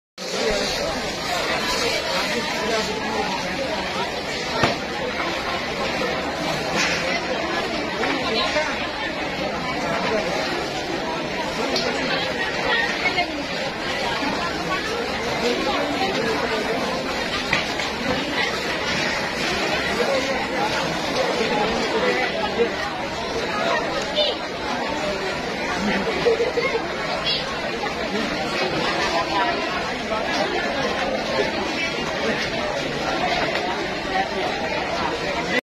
KEBAKARAN DI PIK PENGGILINGAN CAKUNG JAKARTA TIMUR
Telah Terjadi Kebakaran di PIK PENGGILINGAN Blok A, Cakung Jakarta Timur Sekitar Pukul 16.15 wib Jum'at, 24/12/2021.